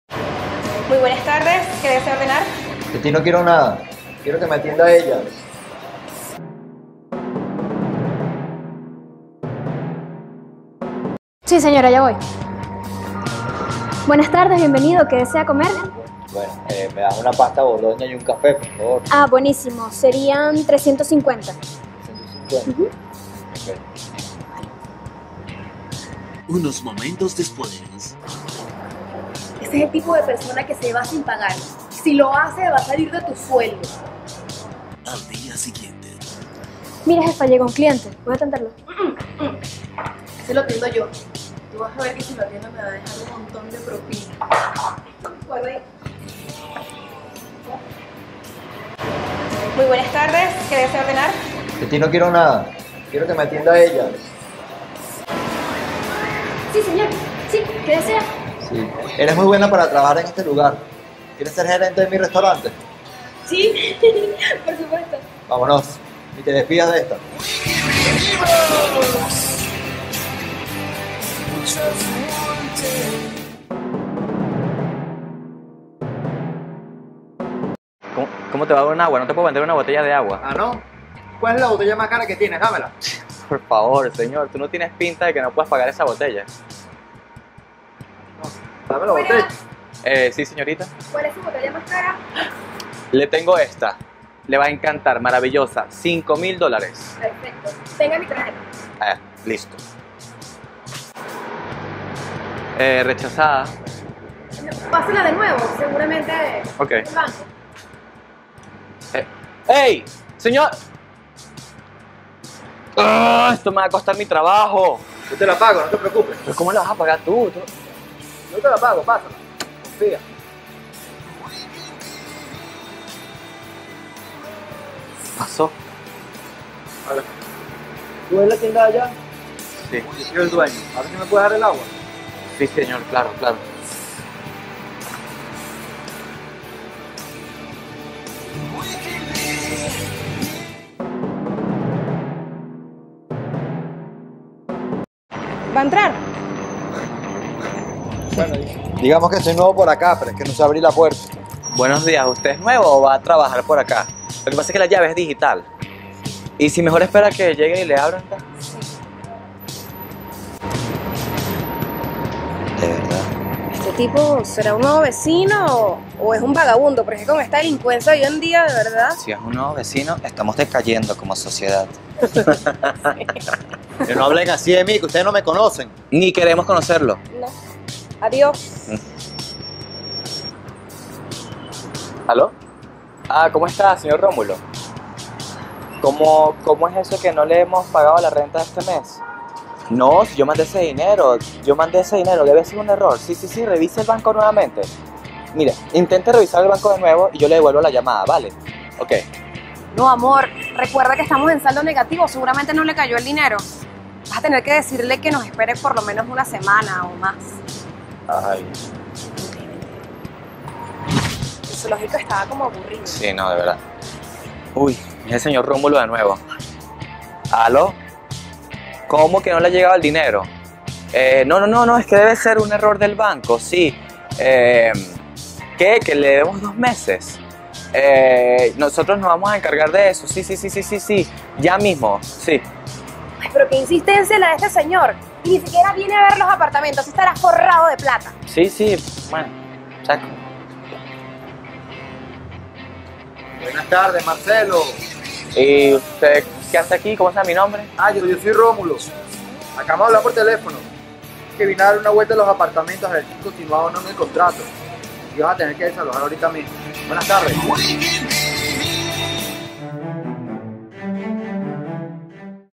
Nunca se debe juzgar a un libro por su portada
Historias de personas que fueron señaladas y juzgadas sin saber quienes eran realmente.